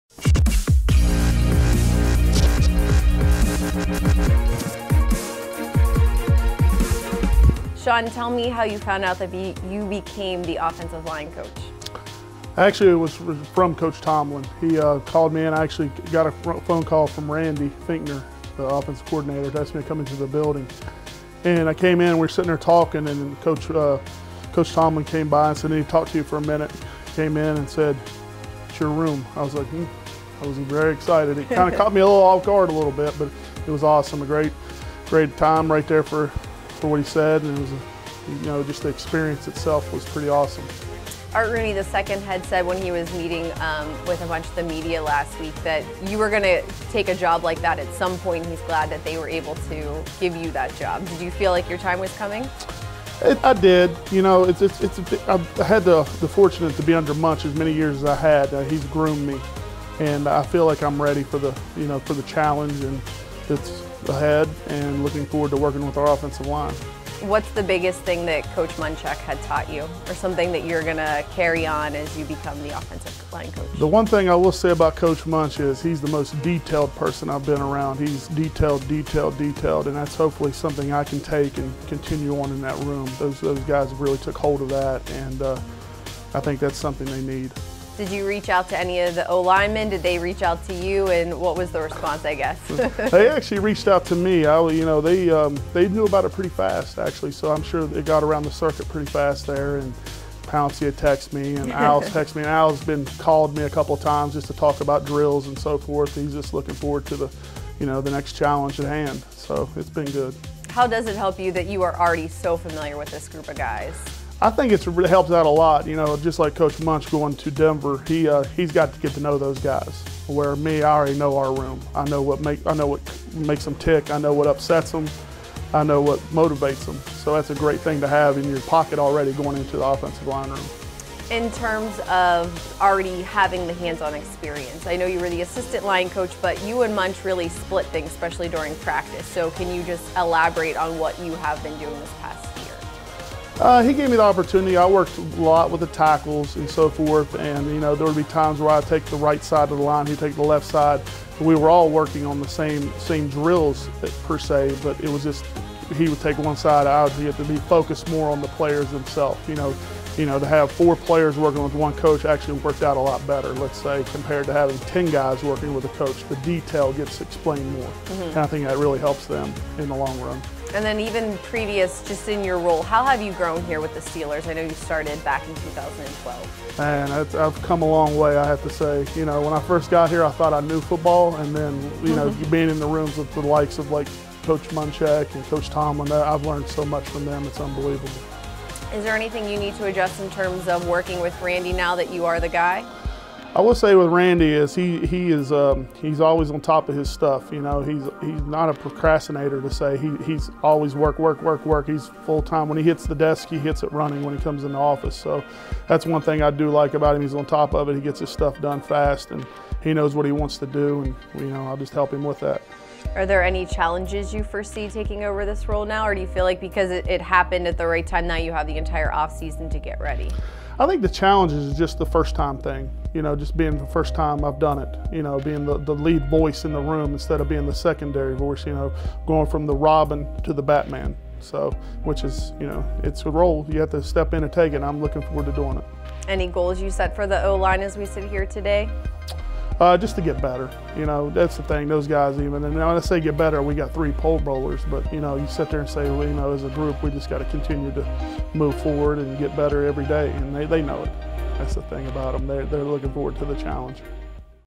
New O-line Coach Shaun Sarrett on the Challenge, Learning from Munchak | Steelers Face Time
The Pittsburgh Steelers new Offensive Line Coach Shaun Sarrett talks about the challenge ahead, learning from Mike Munchak, his familiarity with the guys and more.